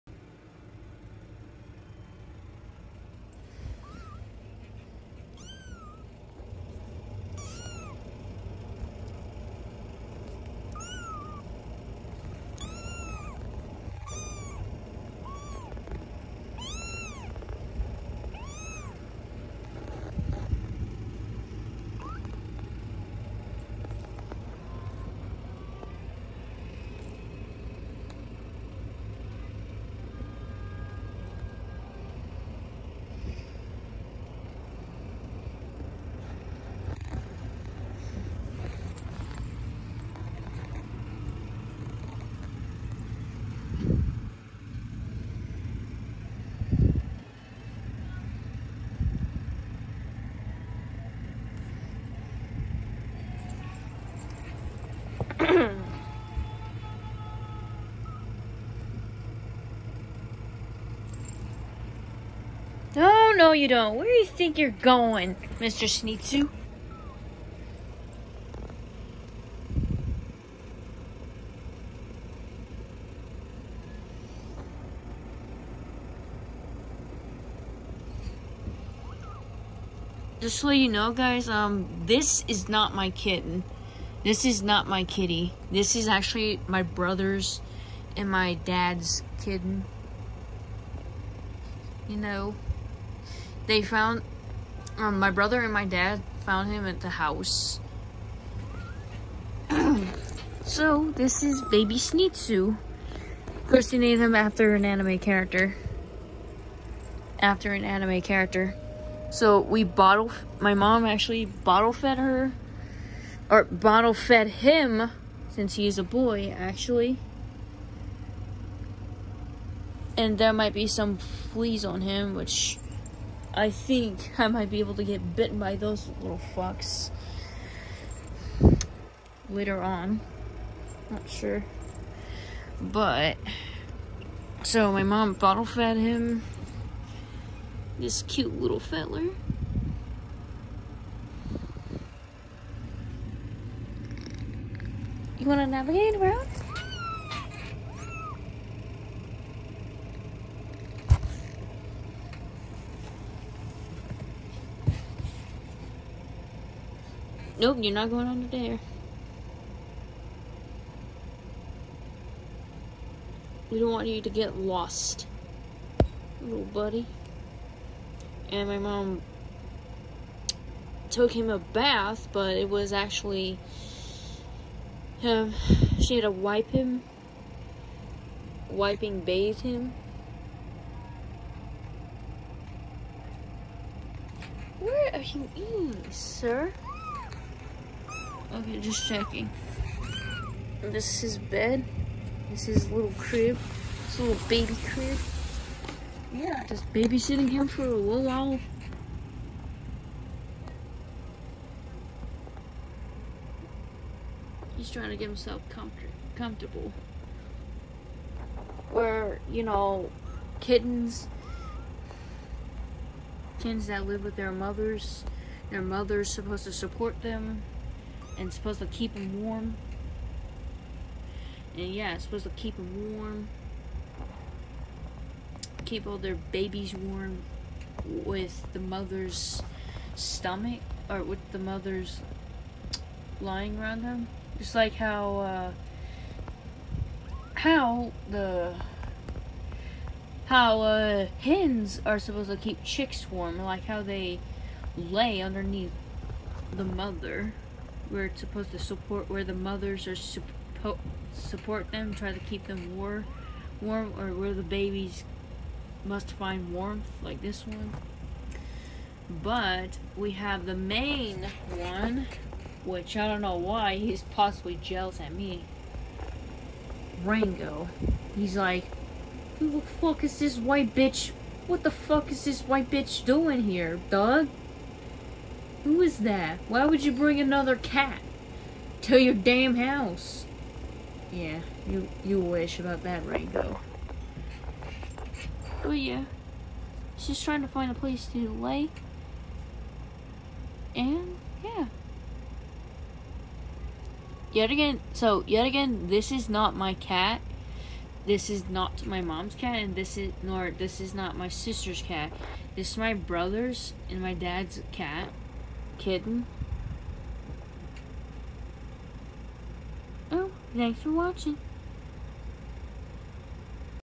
Meet baby Zenitsu
This kitten is not mine nor my mom’s and sister’s.
This kitten belongs to my brother and my dad.
My brother found this kitten at his dad’s house last night.